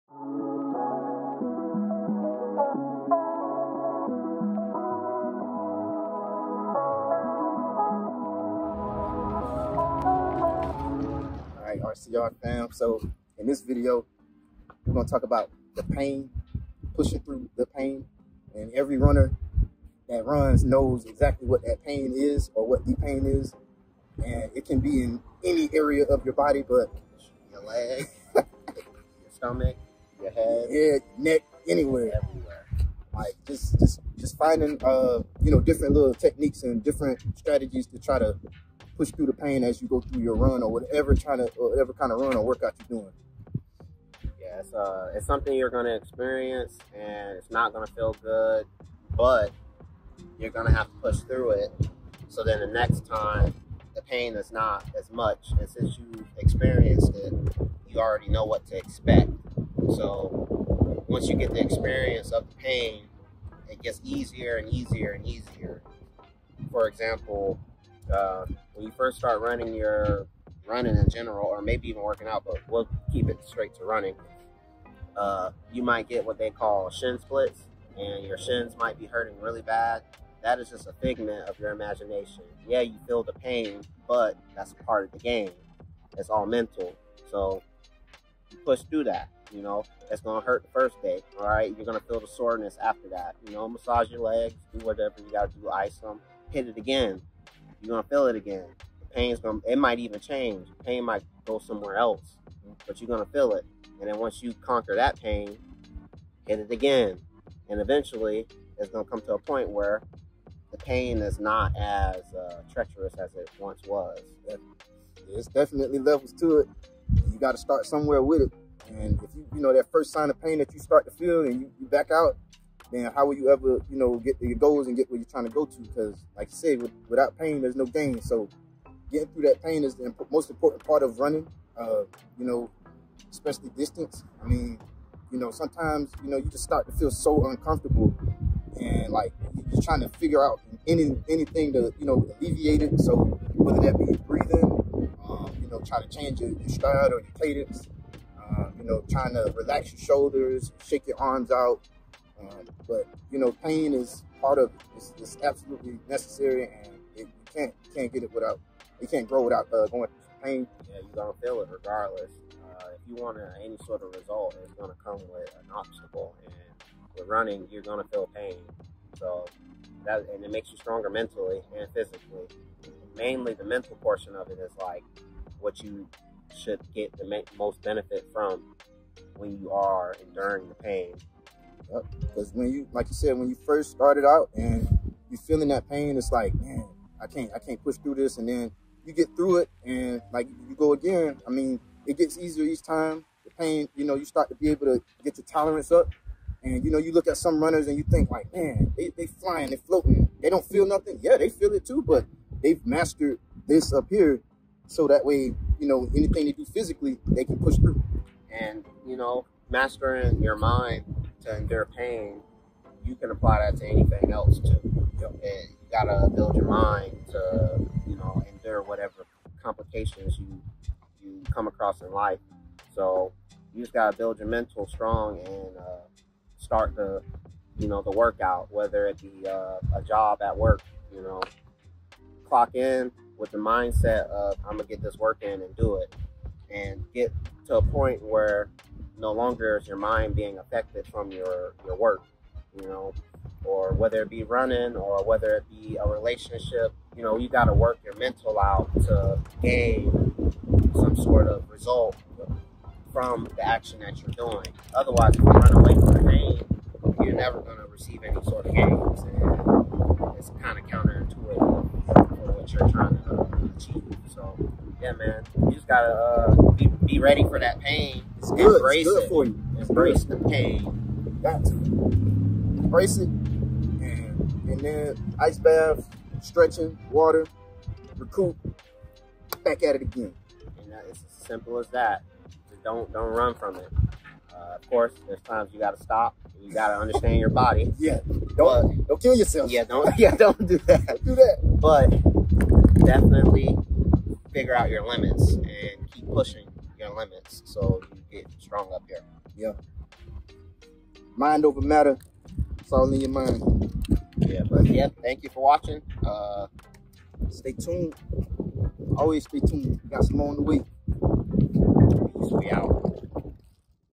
PUSHING THRU THE PAIN | FITNESS & HEALTH TIPS
What’s good RCR Fam! We’re back at it again! And in this video we talk about Pushing Through Pain!

You know, that pain you feel each time you run that kind of lingers and flares up at the worst time? Yeah that pain! The thing is, you’re going to experience it no matter what. And you have to prepare yourself beforehand for pain to come from any body part. It’s how you handle those various agonies that determines how well you perform!

When we feel pain it’s natural to do things to adjust your body and avoid feeling it. You may also feel like giving in because of it, but that’s not an option when you’re out there! So pushing through the pain is vital when running and we talk about some strategies both mentally and physically that can help you push through and finish each run strong. It’s not easy but it’s necessary and doable! And remember, no pain, no gain.